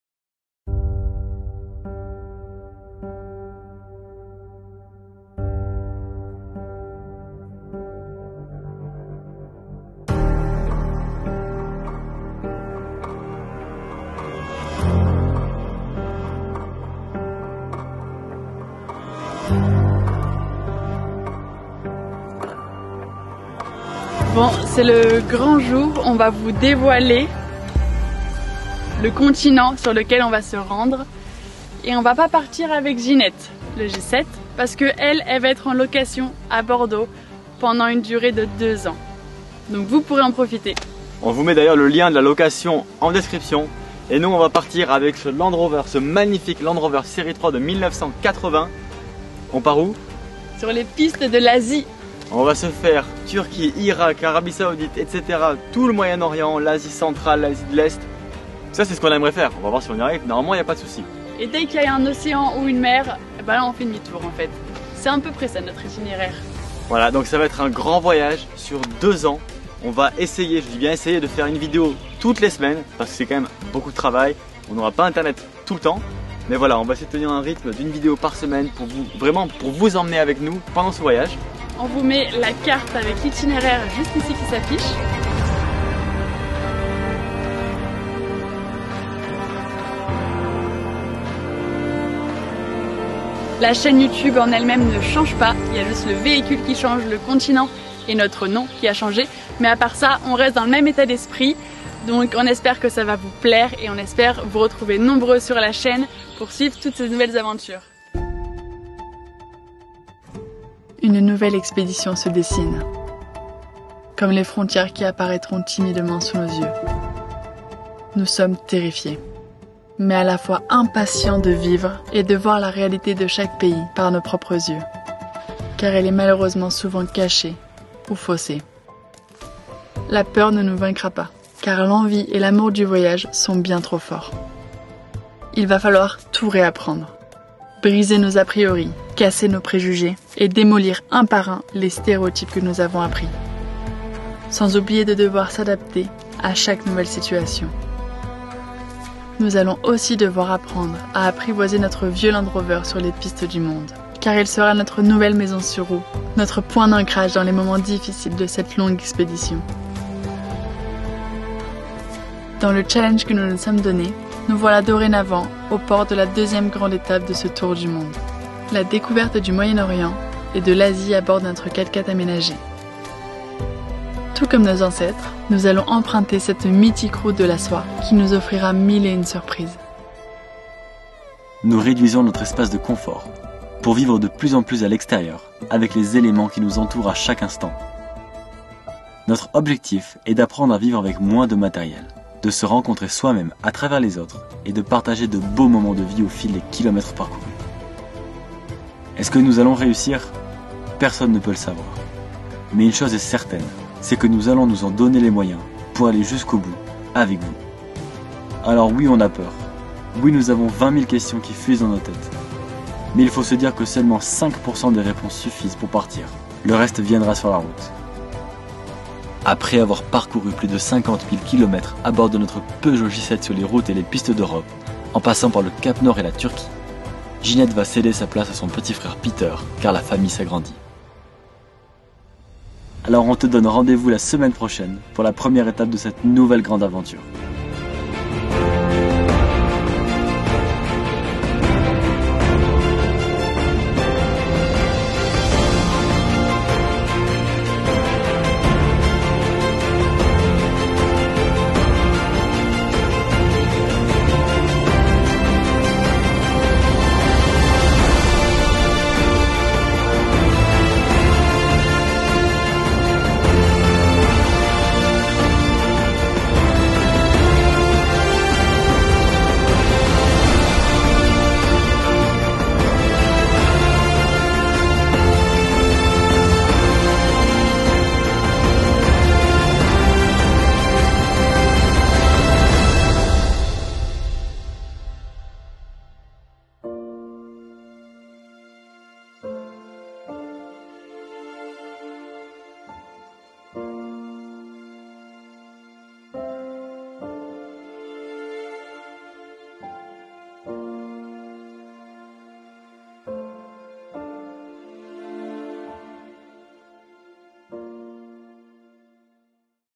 S3E15 : MAIS OÙ PART-ON ?
Dans cette vidéo, on vous dévoile notre nouvel itinéraire pour les deux prochaines années !
Nous partons à la découverte du Moyen-Orient et de l’Asie à bord de notre Land Rover Série 3 !

Alors on te donne rendez-vous chaque semaine pour un nouvel épisode de cette aventure 😉

Matthias et Eva